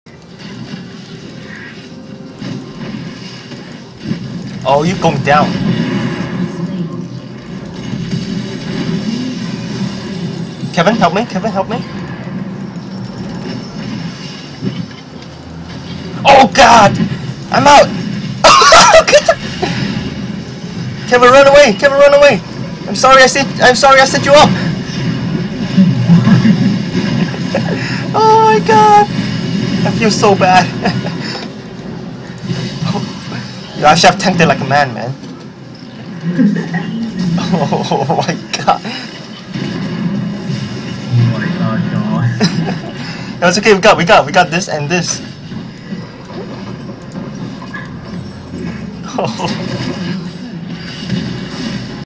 Jonathan Simple - Never Trust Jax
Too lazy to compile the clips together so enjoy the three short Jax clips!